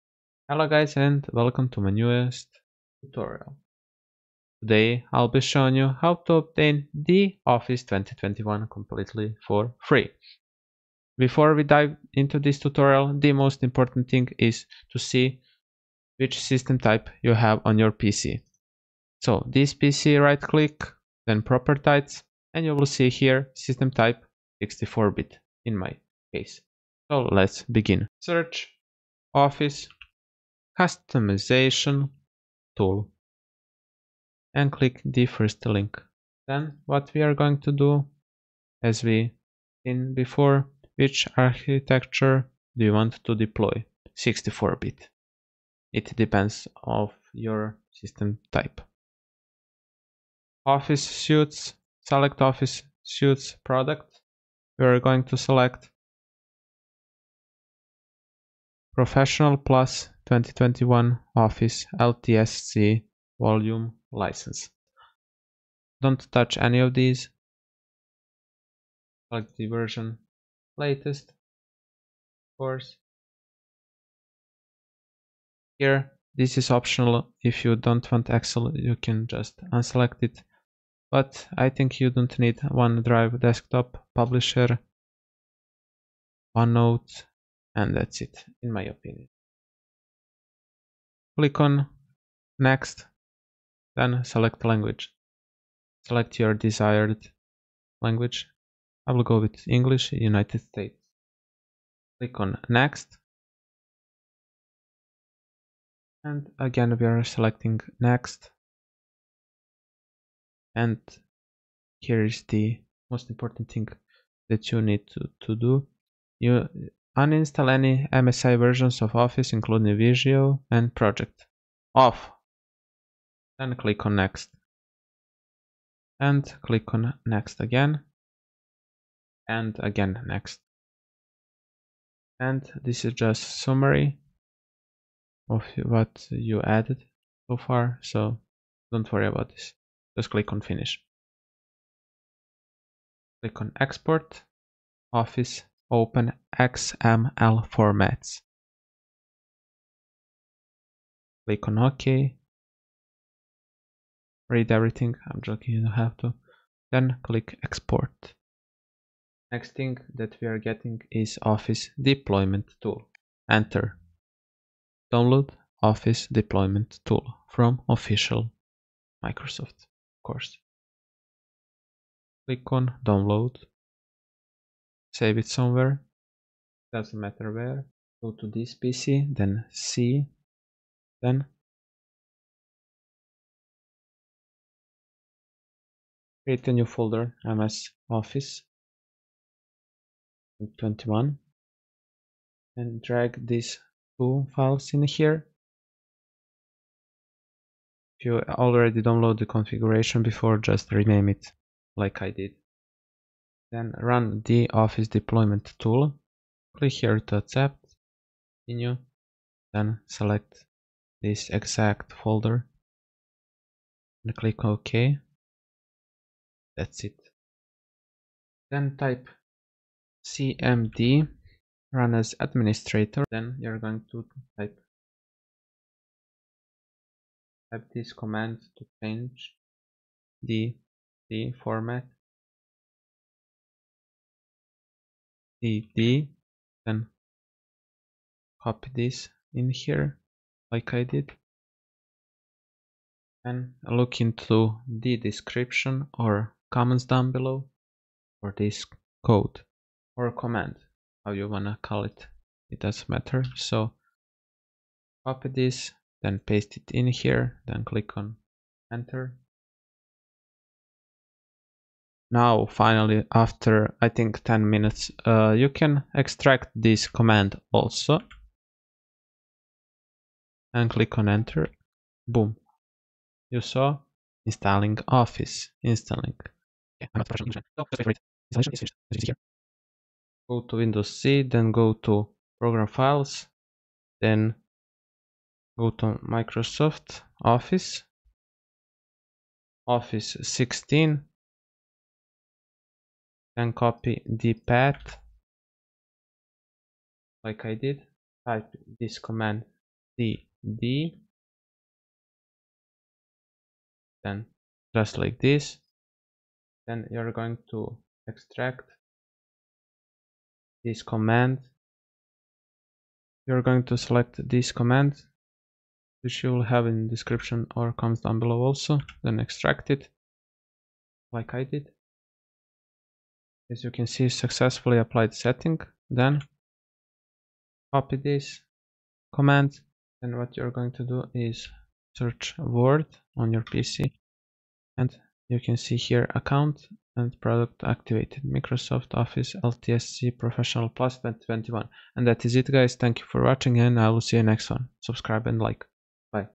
How to Install and Activate Microsoft Office 365 for Free in 2024 (LIFETIME)
Instalation
1) cd C\:MS Office 2021
2) setup.exe /download configuration.xml
3) setup.exe /configure configuration.xml

Activation
4) cd C:\Program Files\Microsoft\Office 16